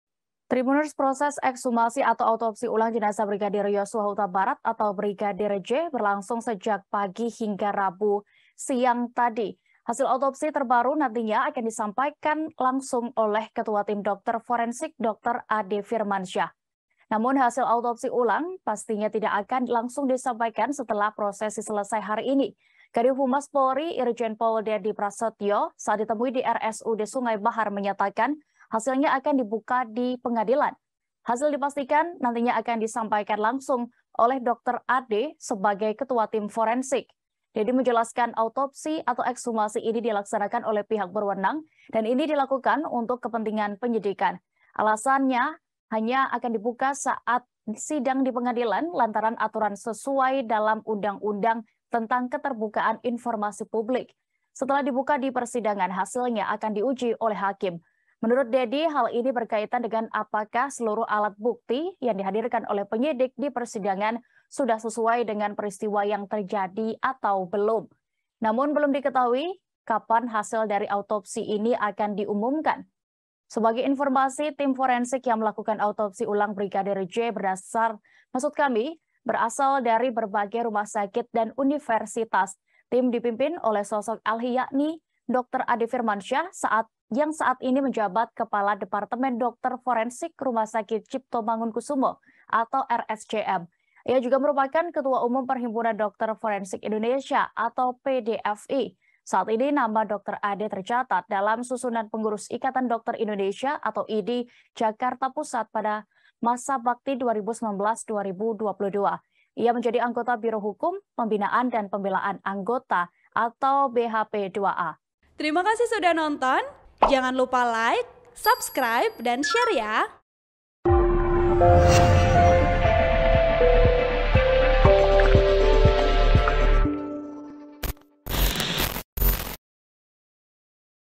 Sosok Dokter Ade Firmansyah, Ketua Tim Forensik yang Akan Sampaikan Hasil Autopsi Ulang Brigadir J
TRIBUN-VIDEO.COM - Proses ekshumasi atau utopsi ulang Jenazah Brigadir Yosua Hutabarat masih berlangsung sejak pagi hingga Rabu (27/7) siang.

Hasil autopsi terbaru nantinya akan disampailkan oleh ketua tim dokter forensik, dokter Ade Firmansyah.

Namun, hasil autopsi ulang itu pastinya tidak akan langsung disampaikan setelah prosesi selesai hari ini.

Kadiv Humas Polri, Irjen Pol Dedi Prasetyo saat ditemui di RSUD Sungai Bahar menyatakan, hasilnya akan dibuka di pengadilan.

Hasil dipastikan nantinya akan disampaikan langsung oleh dokter Ade sebagai ketua tim forensik.

Dedi menjelaskan, autopsi atau ekshumasi ini dilaksanakan oleh pihak berwenang, dan ini dilakukan untuk kepentingan penyidikan.

Alasan hanya akan dibuka saat sidang di pengadilan, lantaran aturan sesuai dalam Undang Undang tentang Keterbukaan informasi publik.

Setelah dibuka di persidangan, hasilnya akan diuji oleh hakim.

Menurut Dedi, hal ini berkaitan dengan apakah seluruh alat bukti yang dihadirkan oleh Penyidik di persidangan sudah sesuai dengan peristiwa yang terjadi atau belum.

Namun belum diketahui kapan hasil dari autopsi akan diumumkan.

Sebagai informasi, tim forensik yang melakukan autopsi ulang Brigadi J berasal dari berbagai rumah sakit dan universitas.

Tim dipimpin oleh sosok ahli yakni Dokter Ade Firmansyah yang saat ini menjabat Kepala Departemen Dokter Forensik Rumah Sakit Cipto Mangunkusumo (RSCM).

Ia juga merupakan Ketua Umum Perhimpunan Dokter Forensik Indonesia (PDFI).

Saat ini, nama Dokter Ade tercatat dalam susunan pengurus Ikatan Dokter Indonesia (IDI) Jakarta Pusat masa bakti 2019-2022.

Ia menjadi anggota Biro Hukum, Pembinaan, dan Pembelaan Anggota (BHP2A).(*)